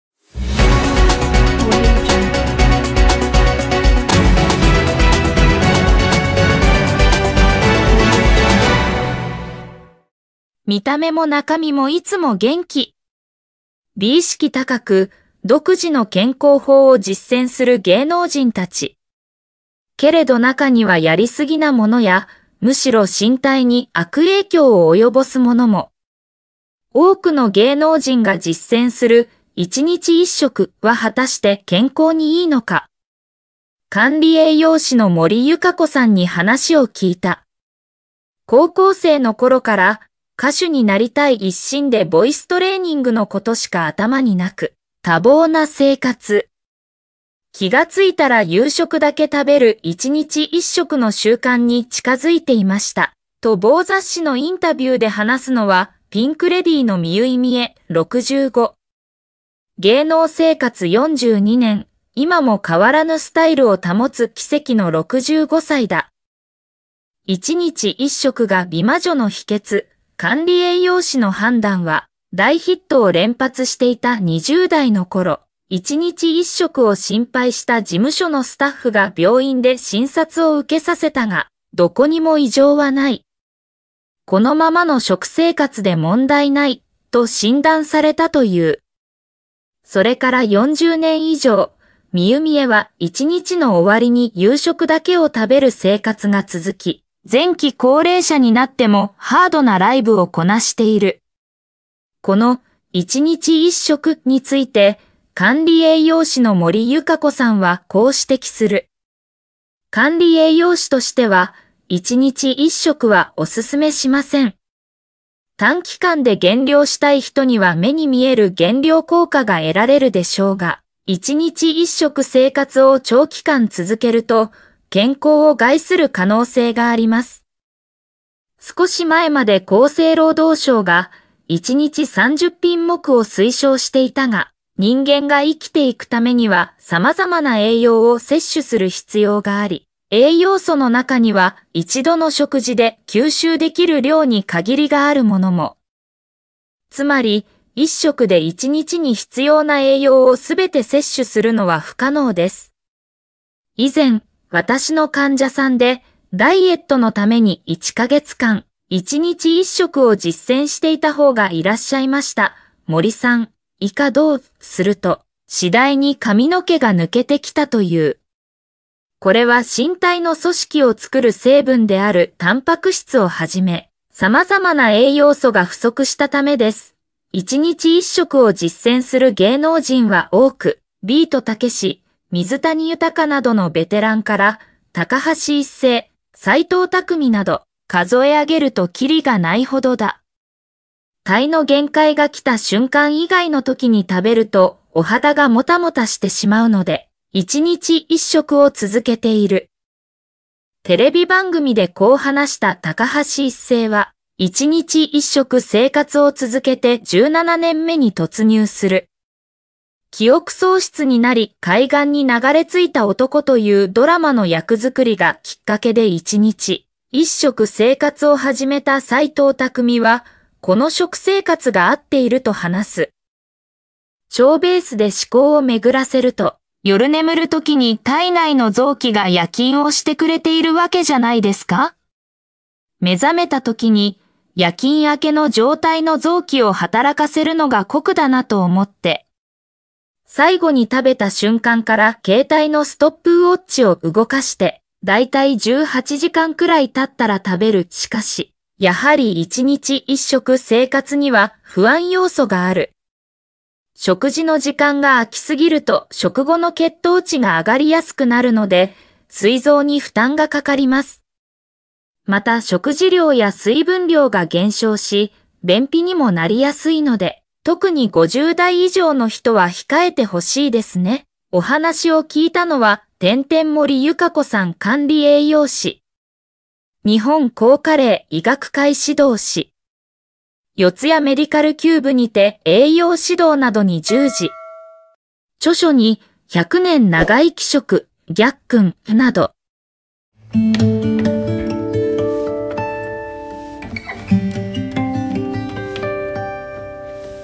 「1日1食」は美の秘訣？芸能人の“危険”な健康法に専門家が待った！「髪の毛が抜けます」 | ニュース 2023年8月14日
「1日1食」は美の秘訣？芸能人の“危険”な健康法に専門家が待った！「髪の毛が抜けます」
見た目も中身もいつも元気! 美意識高く、独自の健康法を実践する芸能人たち。けれど中には“やりすぎ”なものや、むしろ身体に“悪影響”を及ぼすものも。多くの芸能人が実践する「一日一食」は果たして健康にいいのか。管理栄養士の森由香子さんに話を聞いた。
《高校生の頃から、歌手になりたい一心でボイストレーニングのことしか頭になく、多忙な生活。気がついたら夕食だけ食べる一日一食の習慣に近づいていました》

　と某雑誌のインタビューで話すのはピンク・レディーの未唯mie（65）。芸能生活42年、今も変わらぬスタイルを保つ奇跡の65歳だ。

“一日一食”が美魔女の秘訣?管理栄養士の判断は
　大ヒットを連発していた20代のころ、一日一食を心配した事務所のスタッフが病院で診察を受けさせたが、「どこにも異常はない。このままの食生活で問題ない」と診断されたという。

　それから40年以上、未唯mieは一日の終わりに夕食だけを食べる生活が続き、前期高齢者になってもハードなライブをこなしている。この「一日一食」について、管理栄養士の森由香子さんはこう指摘する。

「管理栄養士としては、一日一食はおすすめしません。短期間で減量したい人には目に見える減量効果が得られるでしょうが、一日一食生活を長期間続けると、健康を害する可能性があります」

　少し前まで厚生労働省が「一日30品目」を推奨していたが、人間が生きていくためにはさまざまな栄養を摂取する必要があり、栄養素の中には一度の食事で吸収できる量に限りがあるものも。

「つまり、一食で一日に必要な栄養をすべて摂取するのは不可能です。以前、私の患者さんで、ダイエットのために1か月間、一日一食を実践していた方がいらっしゃいました」（森さん、以下同）

　すると、次第に髪の毛が抜けてきたという。

「これは身体の組織を作る成分であるタンパク質をはじめ、さまざまな栄養素が不足したためです」

　一日一食を実践する芸能人は多く、ビートたけし、水谷豊などのベテランから、高橋一生、斎藤工など、数え上げるとキリがないほどだ。

《体の限界がきた瞬間以外のときに食べるとお腹がモタモタしてしまうので、一日一食を続けている》

　テレビ番組でこう話した高橋一生は、一日一食生活を続けて17年目に突入する。

　“記憶喪失になり海岸に流れついた男”というドラマの役作りがきっかけで一日一食生活を始めた斎藤工は、この食生活が合っていると話す。

《腸ベースで思考をめぐらせると、夜眠る時に体内の臓器が夜勤をしてくれているわけじゃないですか? 目覚めた時に、夜勤明けの状態の臓器を働かせるのが酷だなと思って。最後に食べた瞬間から携帯のストップウオッチを（動かして）、だいたい18時間くらいたったら食べる》

　しかし、やはり一日一食生活には不安要素がある。

「食事の時間が空きすぎると食後の血糖値が上がりやすくなるので、膵臓に負担がかかります。また食事量や水分量が減少し、便秘にもなりやすいので、特に50代以上の人は控えてほしいですね」

お話しを聞いたのは……森 由香子さん●管理栄養士。日本抗加齢医学会指導士。四谷メディカルキューブにて栄養指導などに従事。著書に『100年長生き食』（Gakken）など